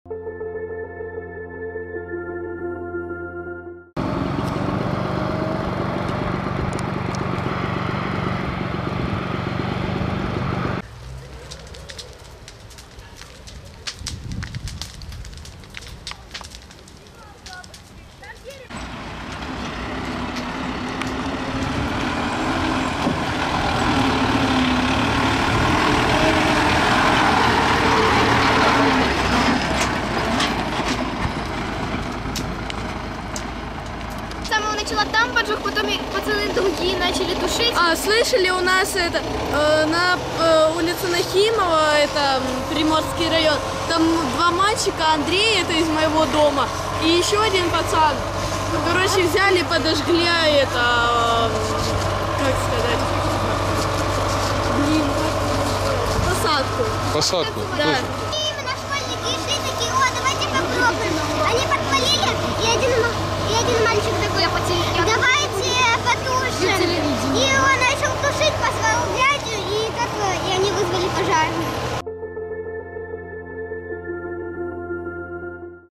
Мариупольские школьники чуть не сожгли микрорайон
В Мариуполе на 23-м микрорайоне на улице Гранитной загорелась сухая трава. По словам местных жителей, траву подожгли школьники.